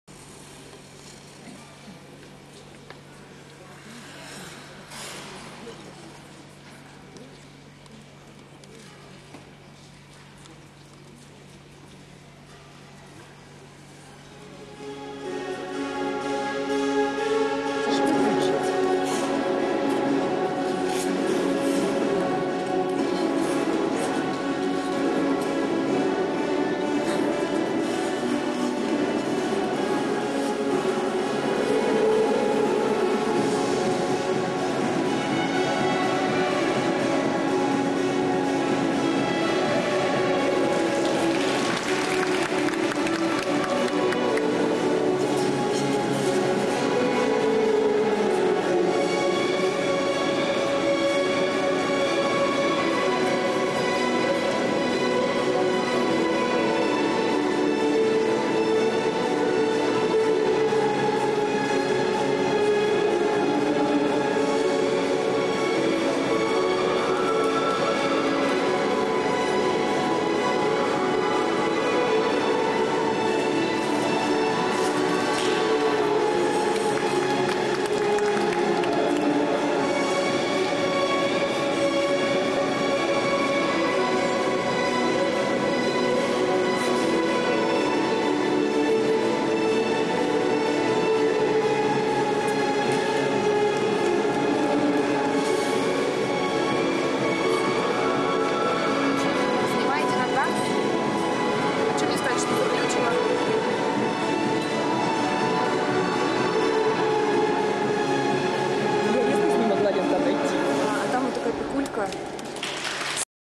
Евгений Михайлов.mpg
Уральская весна 2010. Соревнования фигуристов непрофессионалов.